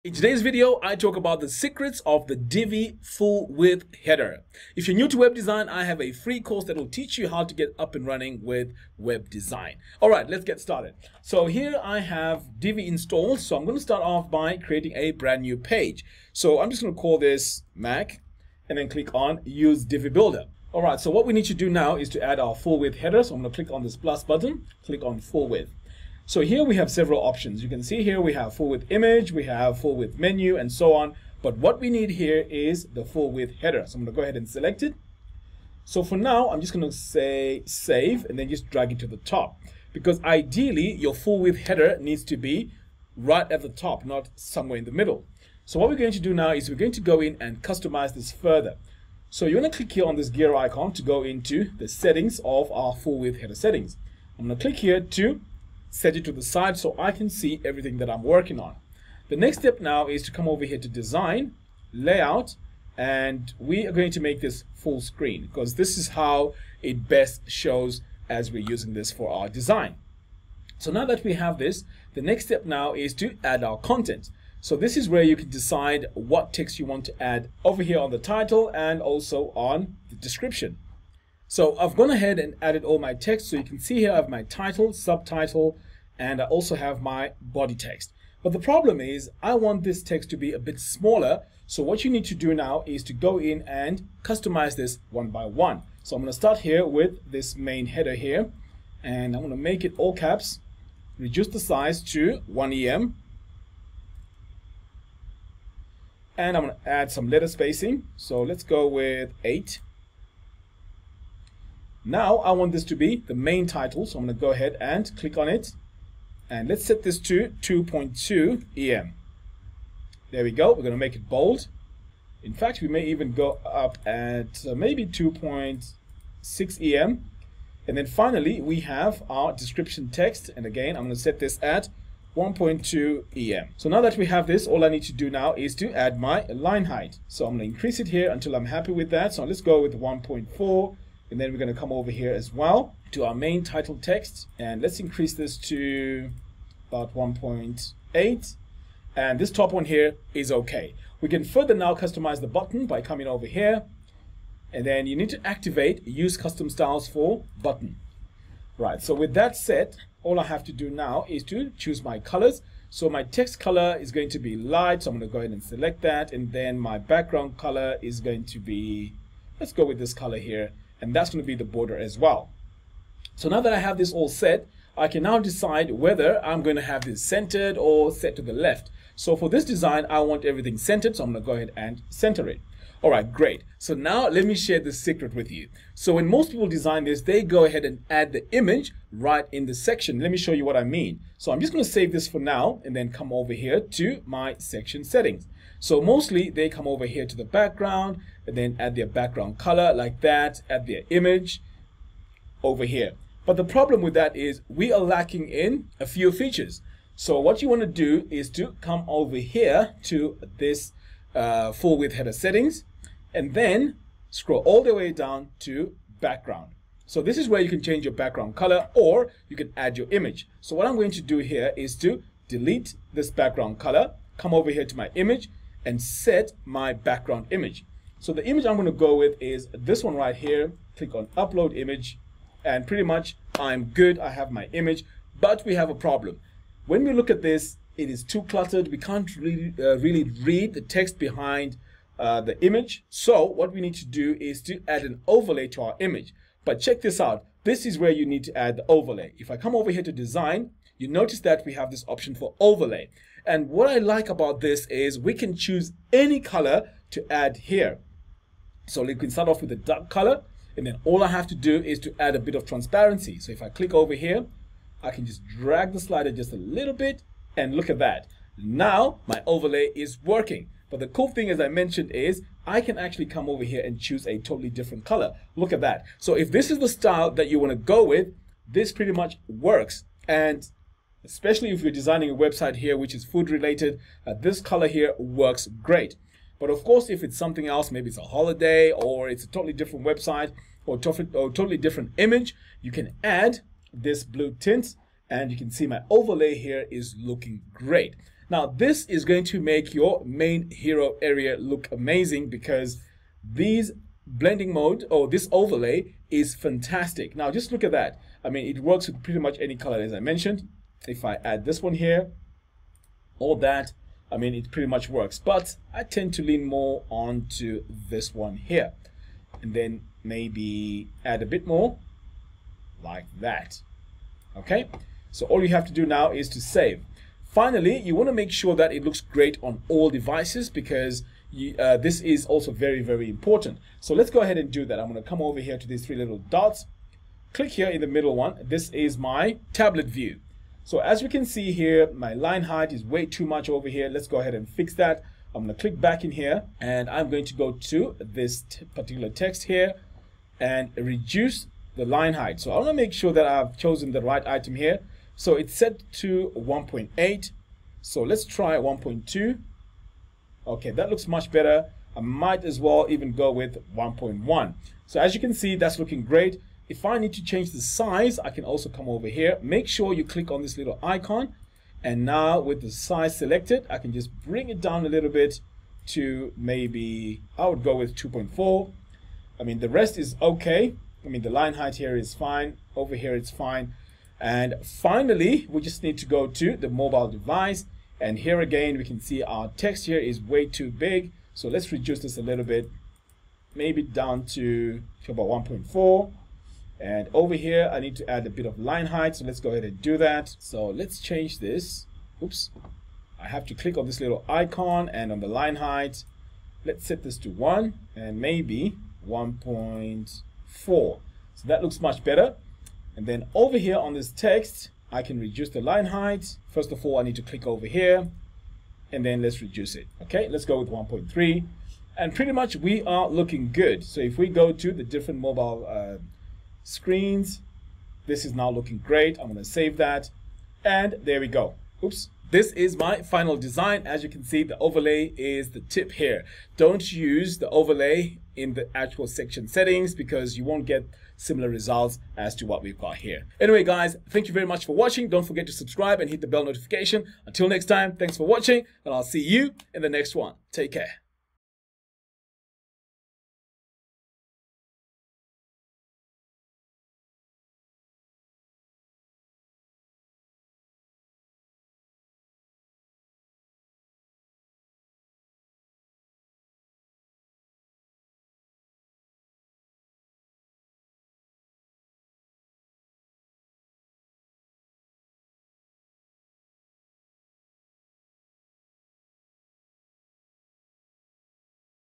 Secrets Of The Divi Full-Width Header - Divi Tutorial
In this video, I talk about a quick Divi tip in the full-width header module that makes you create awesome Divi heroes.